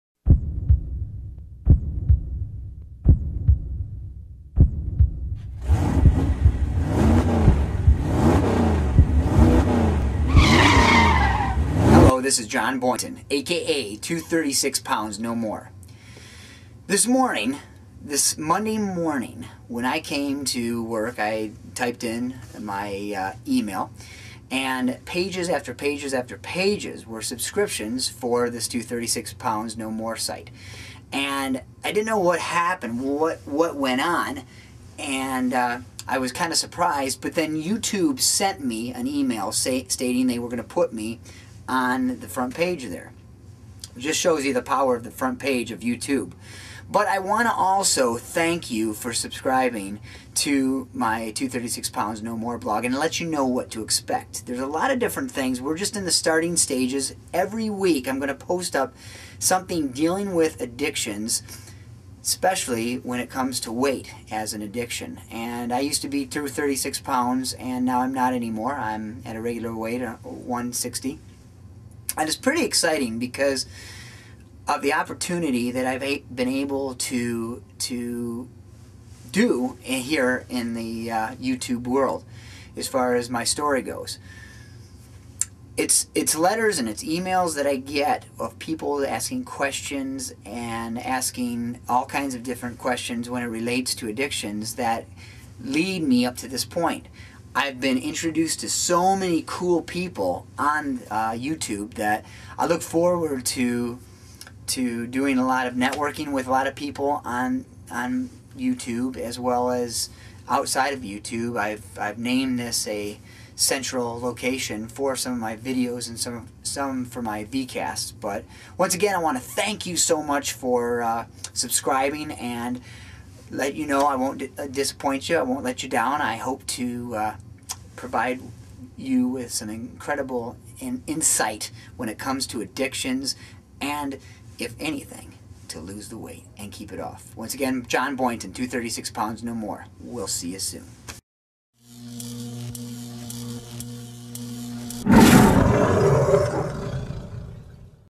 Thank you Subscribers from Jon B
Jon Boynton aka 236lbsnomore is over overwhelmed, overjoyed, and very greatful for all the help from You Tube and all the Subscribers for a cause I believe in. This Special Editorial is for you!